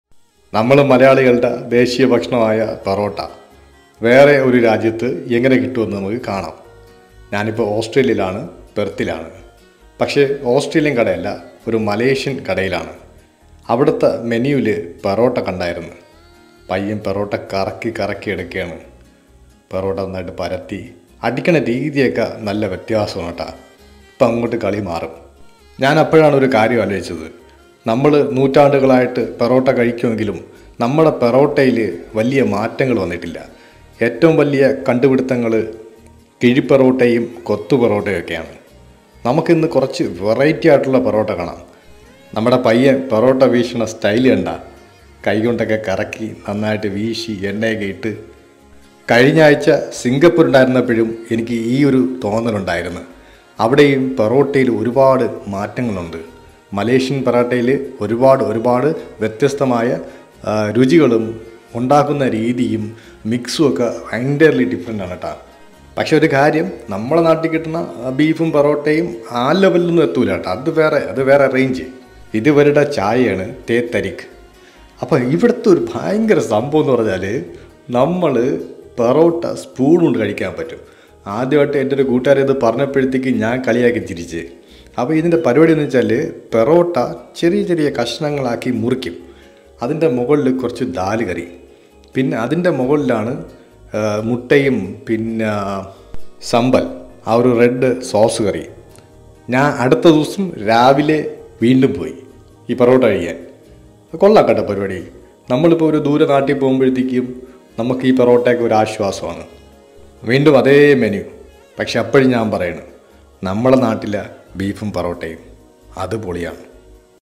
ഓസ്‌ട്രേലിയയിൽ പെറോട്ട കഴിക്കാൻ പോയ മലയാളി യുവാവ് കണ്ട ഞെട്ടിക്കുന്ന കാഴ്ച.
ഓസ്‌ട്രേലിയയിൽ പെറോട്ട കഴിക്കാൻ പോയ മലയാളി യുവാവ് കണ്ട ഞെട്ടിക്കുന്ന കാഴ്ച. നമ്മുടെ പെറോട്ട നമ്മളെ വിട്ടു പോകുകയാണോ ?